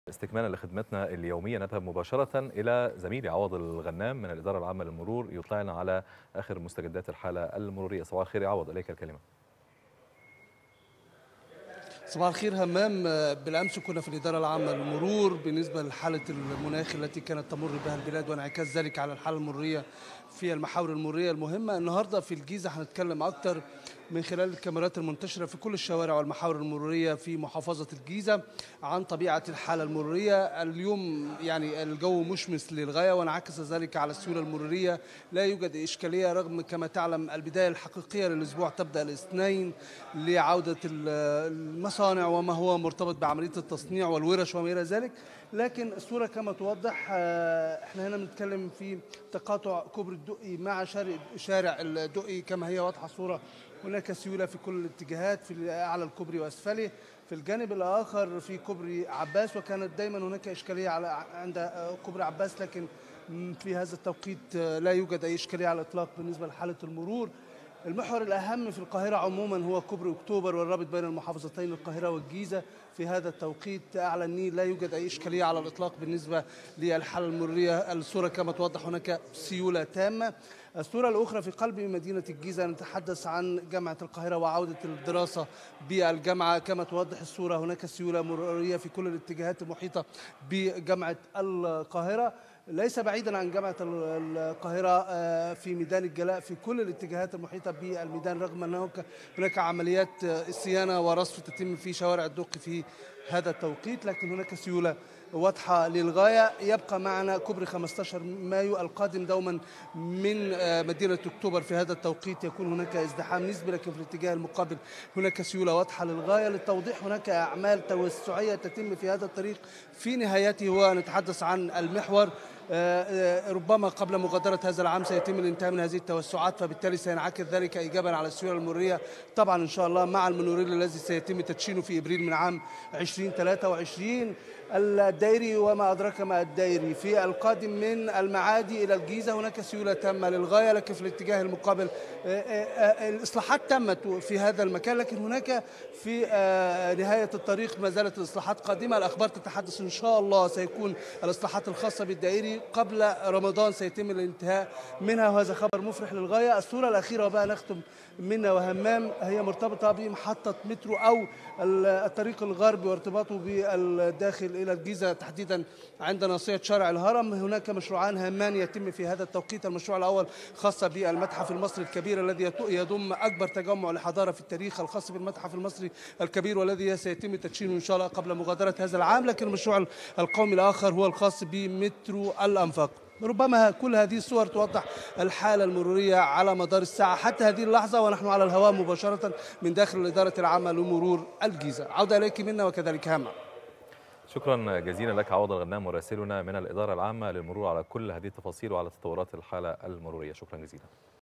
هذا الصباح| إكسترا نيوز ترصد الحالة المرورية من غرفة عمليات الإدارة العامة لمرور الجيزة
21 فبراير 2022